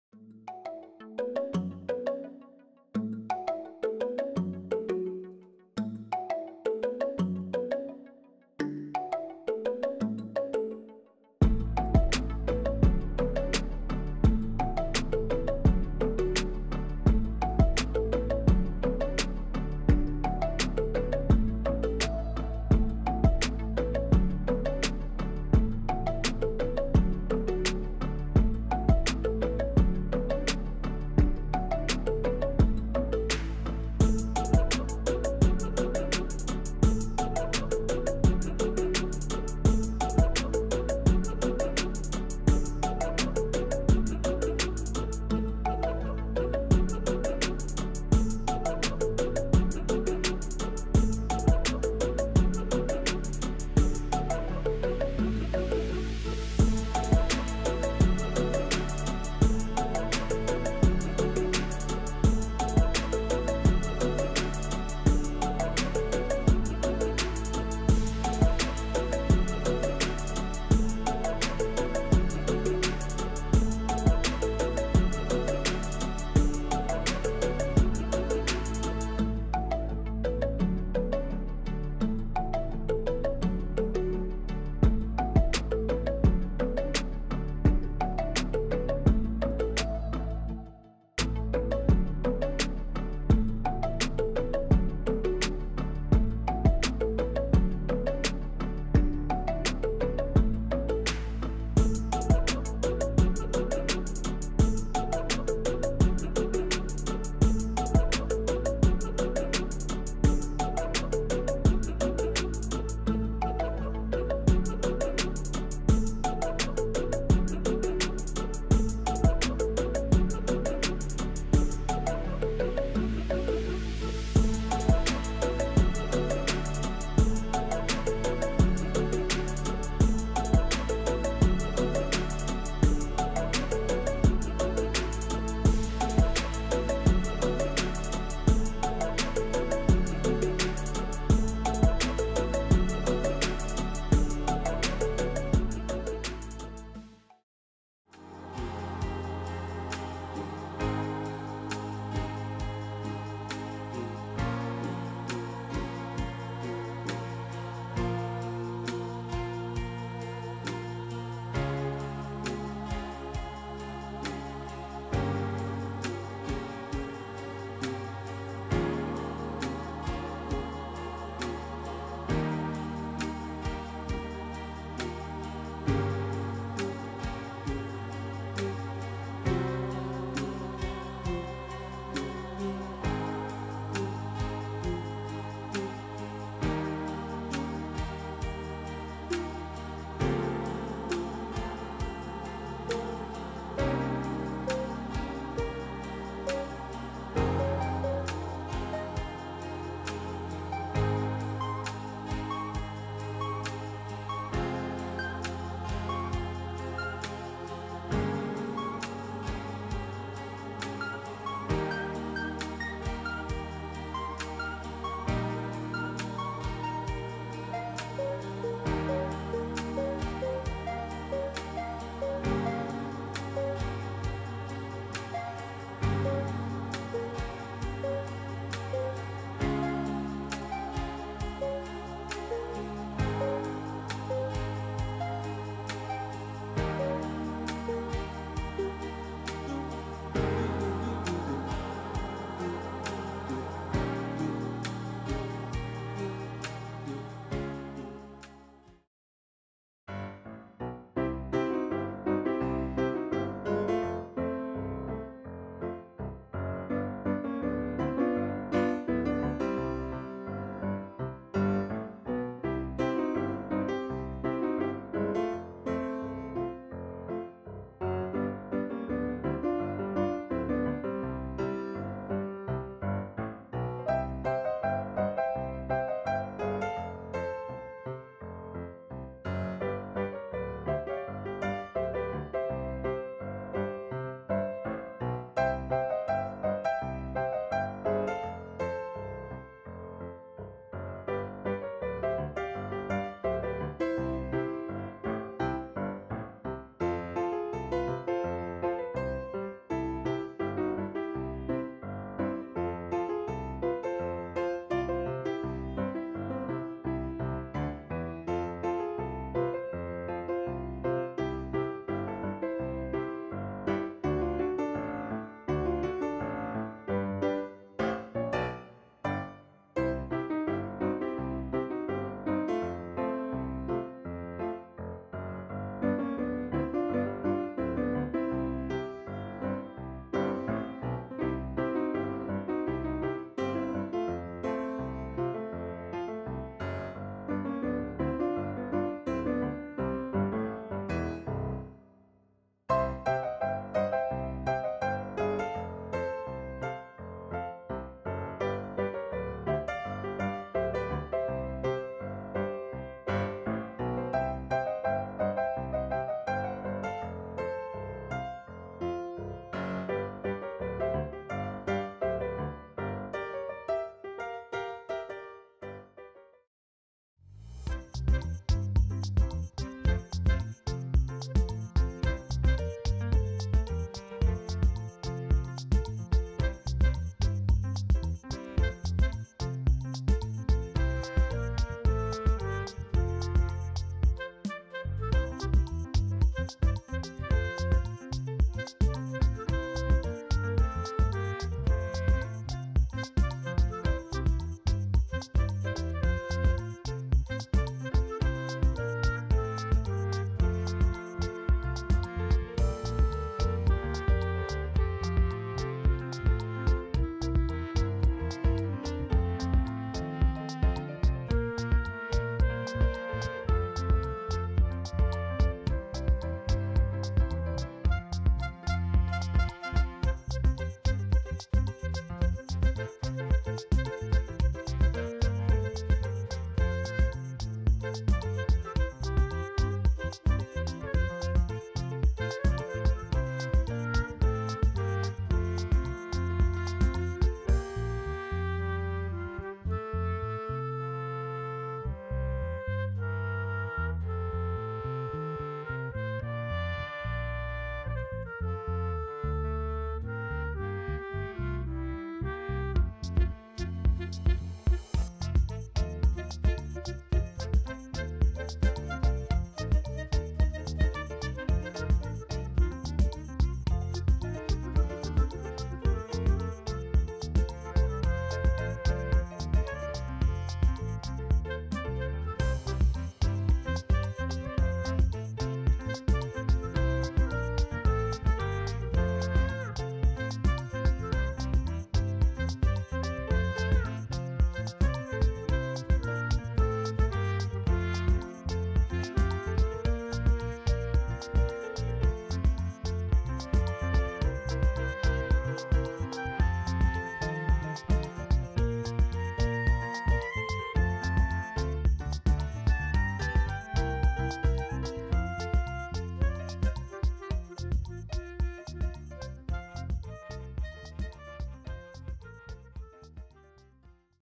Hilarious Cat Memes for Feline Fanatics (PART 26)
😆Do you want have fun time ? Our video "Funny Cat Memes" will brings you a side-splitting compilation of hilarious cat moment.